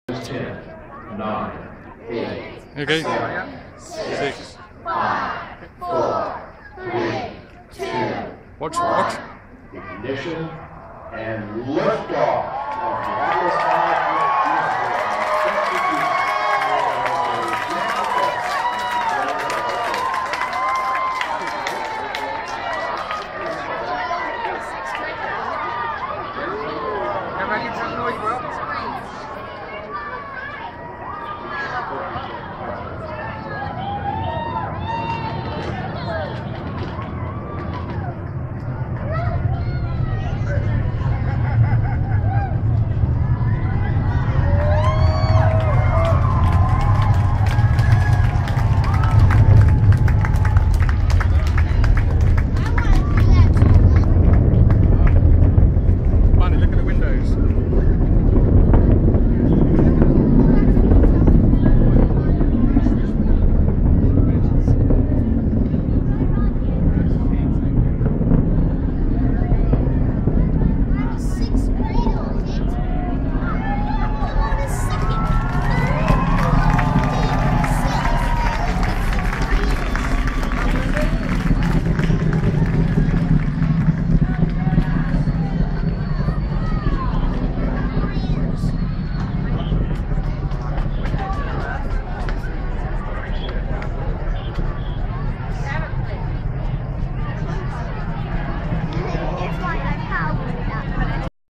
Juno Atlas 5 Rocket Launch
While we were on holiday we were lucky enough to get into the Kennedy Space Center and watch the rocket take off. It was an early start to make sure we got into the viewing area. First rocket launch I have seen and it was very impressive. The sound made the windows wobble in the Saturn V Center.